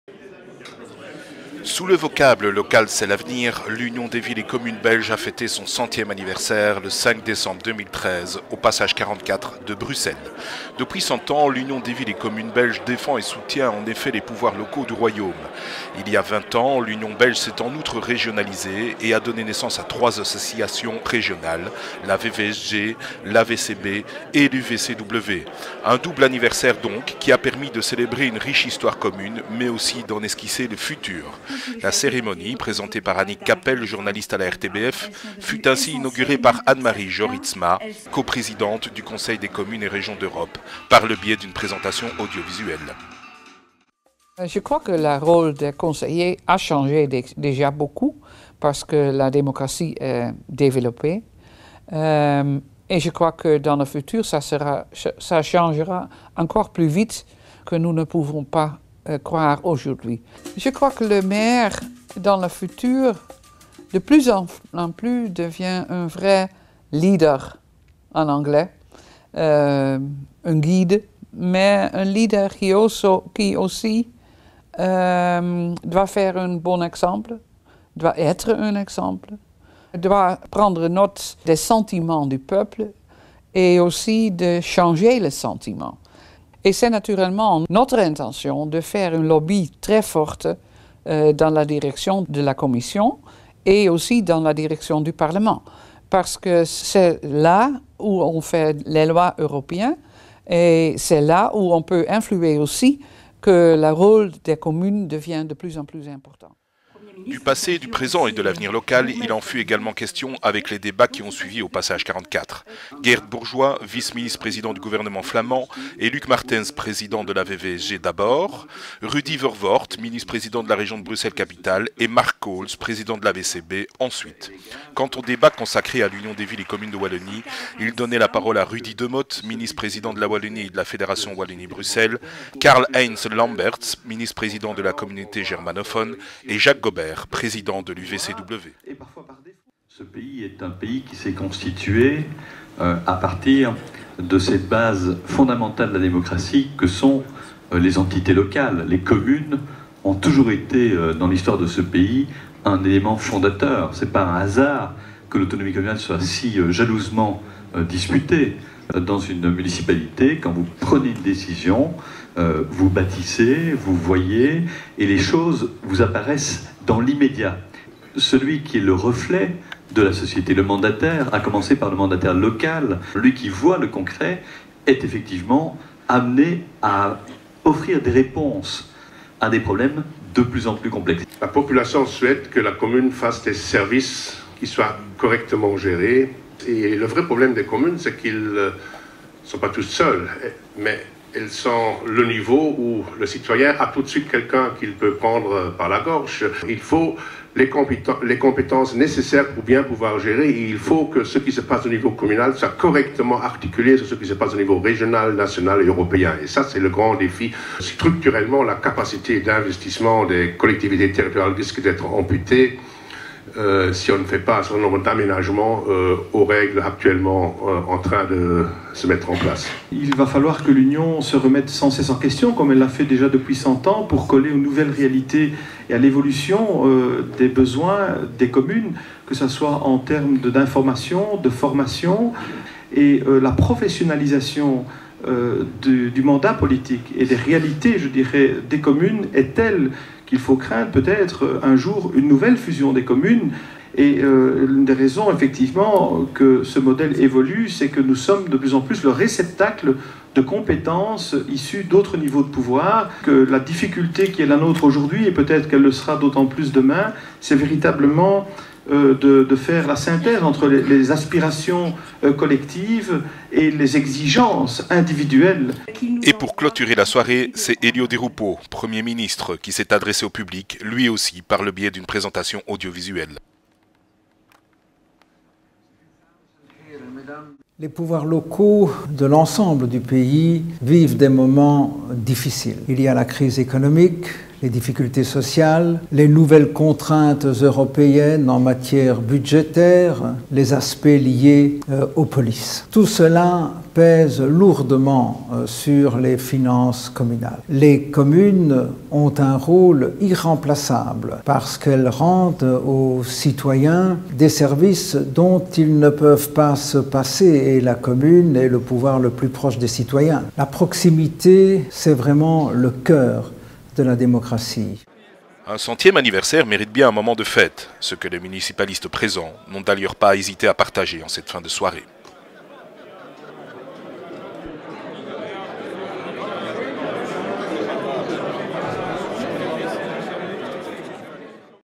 100 ans UVCB
Un reportage sur le 100ème anniversaire de l'UVCB (20 ans de l'UVCW, de la VVSG et de l'AVCB) au Passage 44 de Bruxelles le 5 décembre 2013.